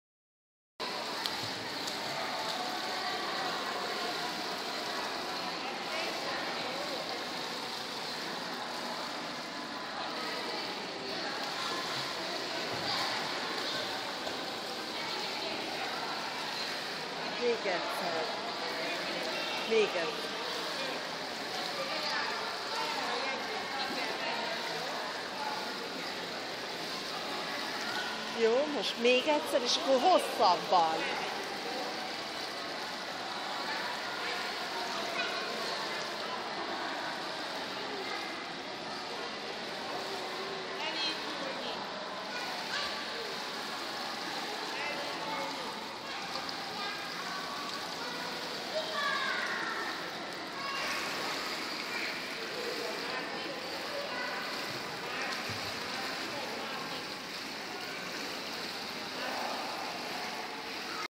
Levi már tud úszni
Levi megtanult úszni az algyői Borbála fürdőben. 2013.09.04.